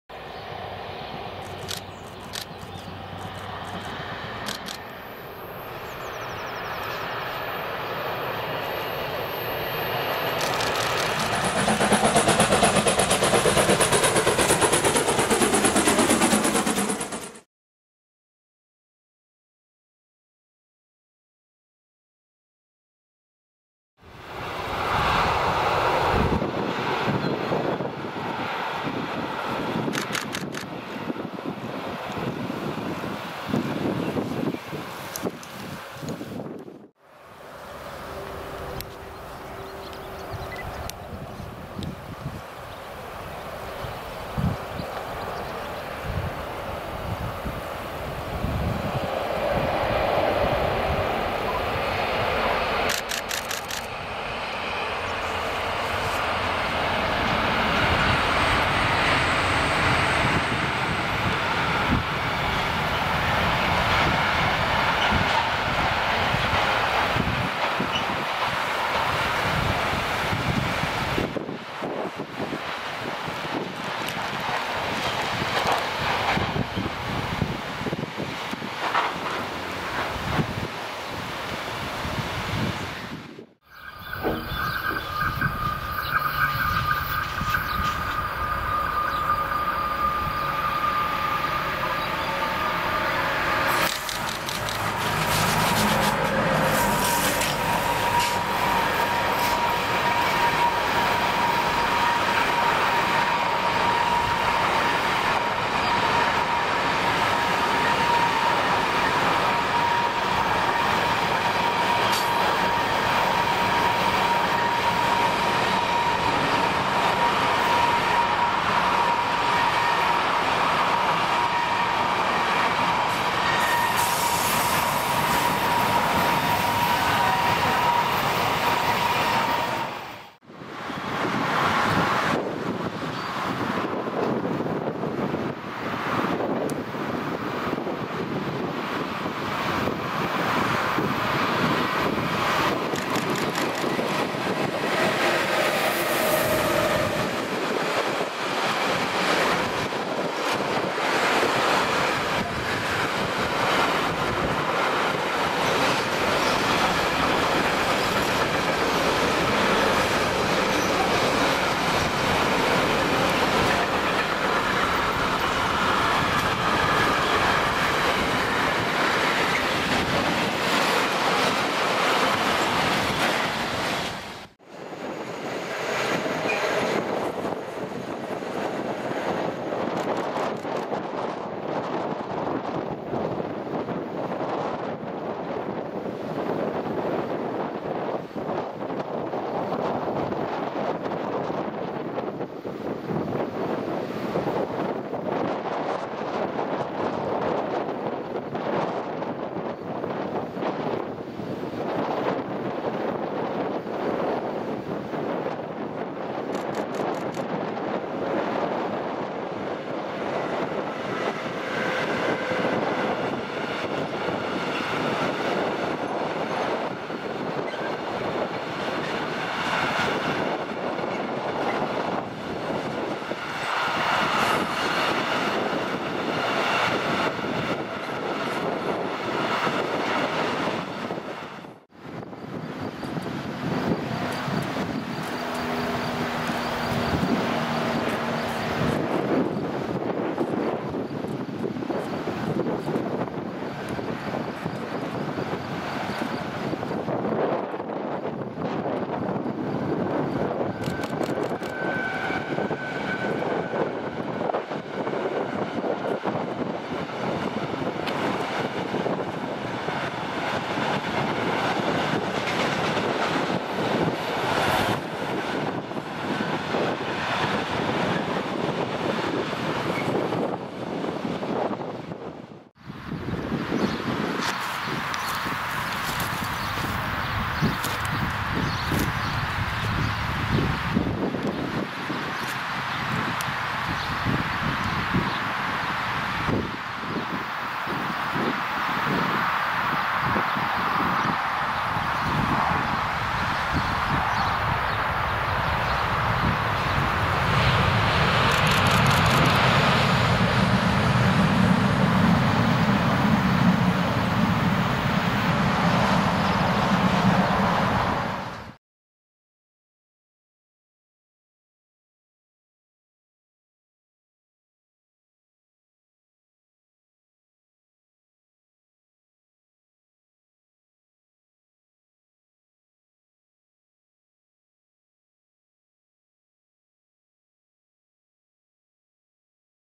Treni a Spiez - 23 e 24 Giugno 2020 - Parte 5 - Dal Thunersee a Faulensee
Una serie di 2 giornate, il 23 e 24 di Giugno del 2020, raccontate attraverso i video ripresi tra Kandersteg e Spiez.
Nella quinta parte ripartiamo da Spiez con una serie di transiti sul rettilineo che porta ad Einigen.
Tra questi un invio di RBDe565 per Thune una doppia di Re425 su un merci intermodale e una E486 con un secondo merci che riprendiamo successivamente a Reichenbach im Kandertal con in testa una Re485.
Fatto questo, dopo il passaggio di un invio di Re425 per Spiez, riprendiamo una coppia mista E486 e E193 BLS con un treno per Novara Boschetto, seguito a ruota da una coppia di E484 SBB Cargo Int.
Infine ci spostiamo a Faulensee dove riprendiamo un treno regionale per Zweisimmen proveniente da Interlaken Ost, eccezionalmete con la Re420 lato Zweisimmen.

Buona visione